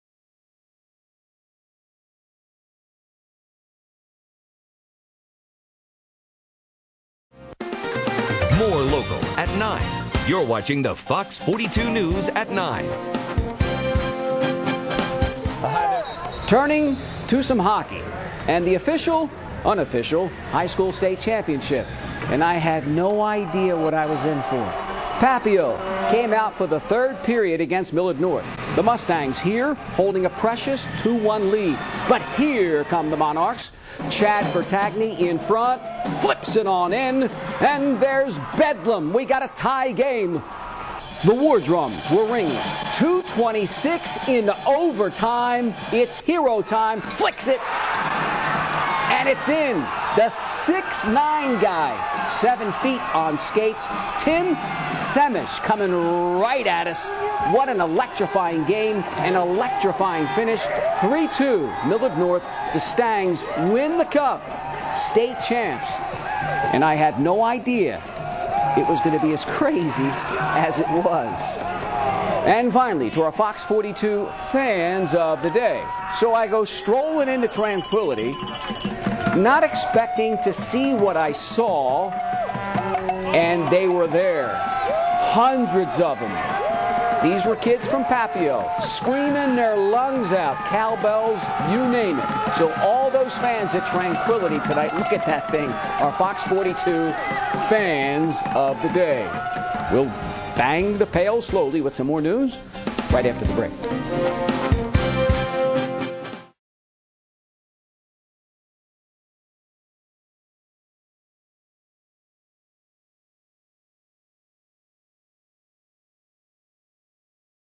2010 Nebraska State High School Championship Hockey game
Exciting highlights from the 2010 O.M.A.H.A. High School Hockey League Championship game: Millard North vs. Papillion held on Thursday March 28, 2010.